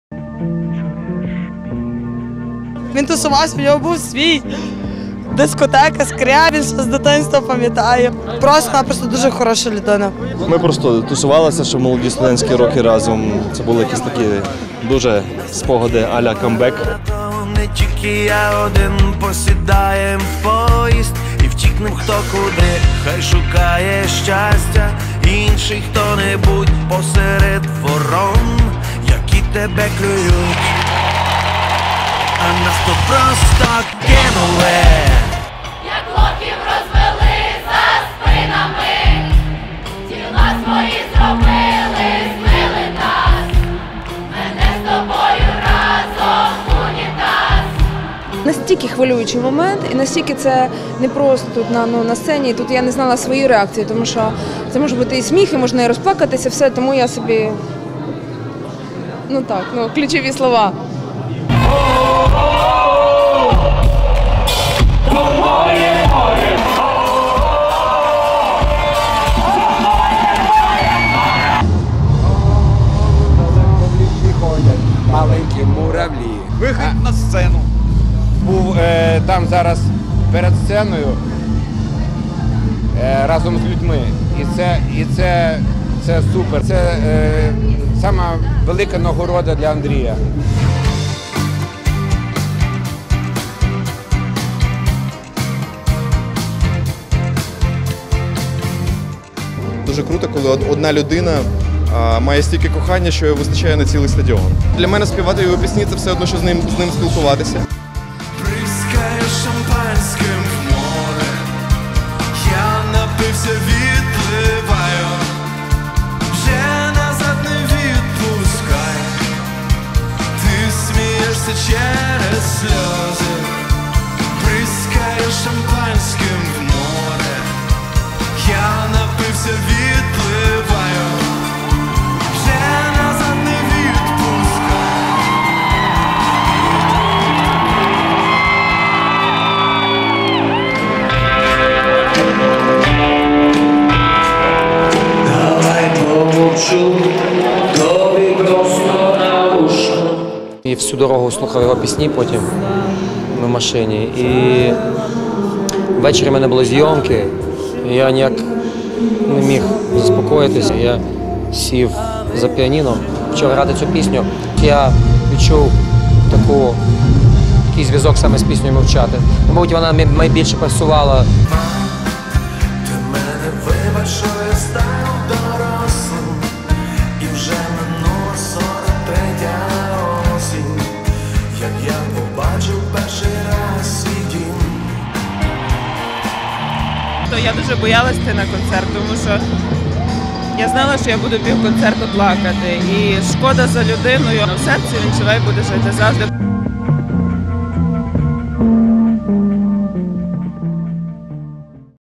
У Львові відбувся концерт пам'яті Кузьми
Десятки тисяч людей прийшли на концерт пам`яті Кузьми «Скрябіна» у Львові